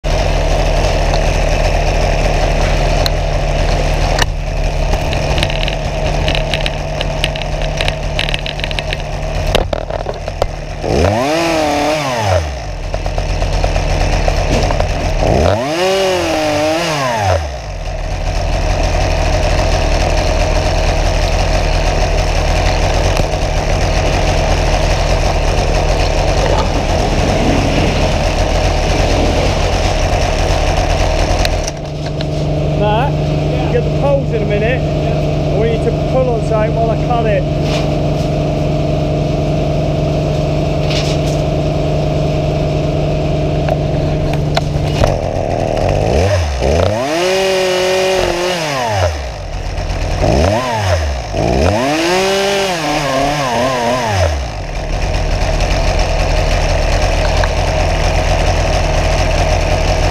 Cherry plum reduction
Go pro attached to chainsaw whilst pruning.